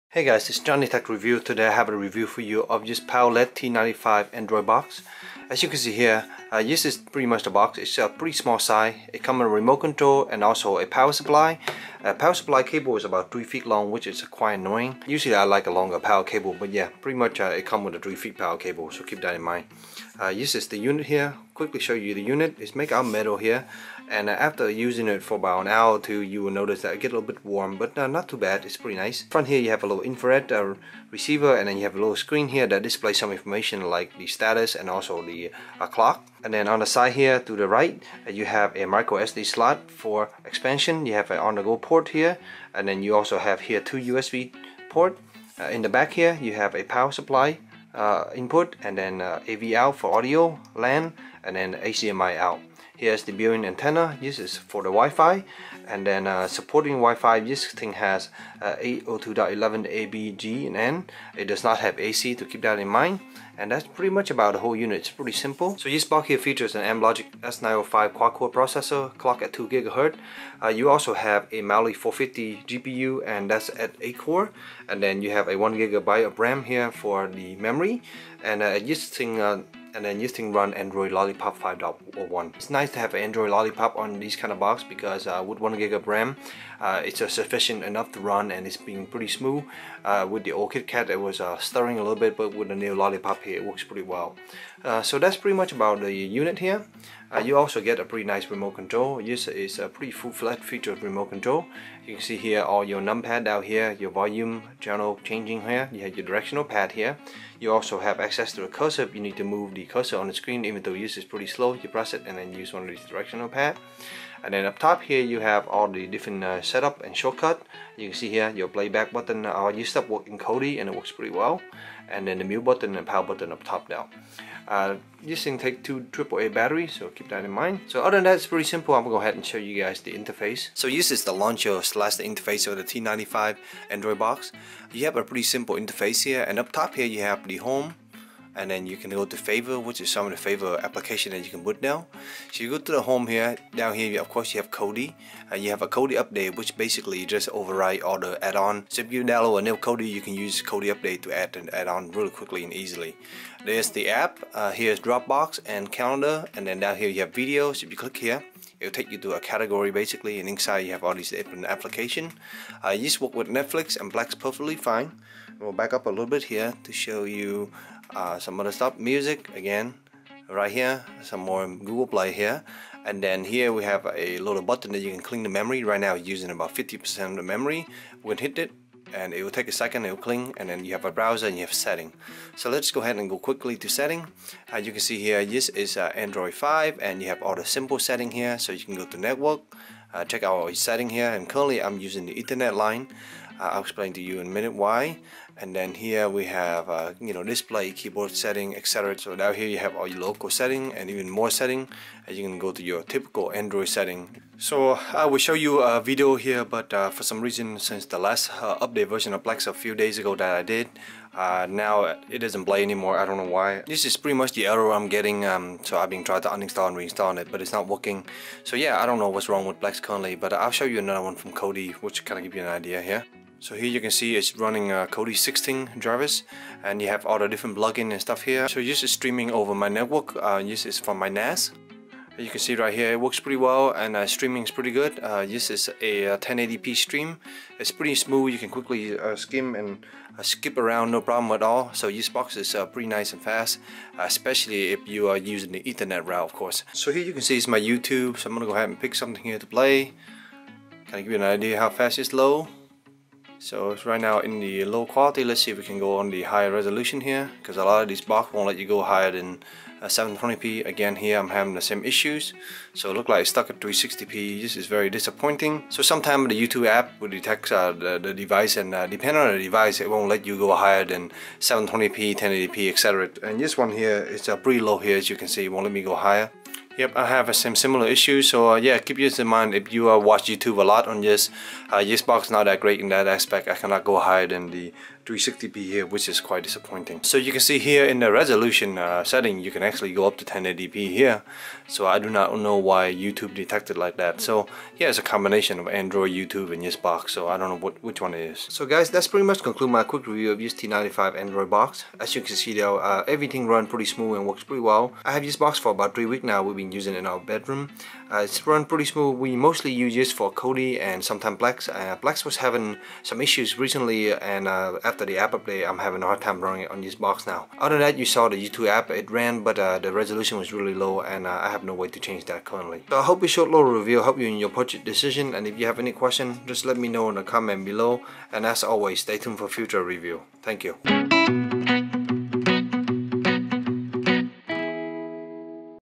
PowerLead T95 Android Quad-core TV Box REVIEW
- Run Kodi well, browsing was smooth with no issues
- Control is easy to use, plenty of option and quick access button. Initial setup I plug in a keyboard to quickly enter all my info.
- Wireless works fine even for streaming but with Plex original quality I ended up using a wire connection for more reliability.
- Plex initially ran find but after the latest software update, that broke it
- Youtube app detect this box and only allow 360p, no way to increase this as far as I know which is disappointed if you plan to use Youtube
Overall the box is speedy and works well with Lolipop 5.0.1 even with only 1 GB of ram. If you plan to use youtube or plex, get another box as this one has some issue currently and I don't think the youtube will ever be fix.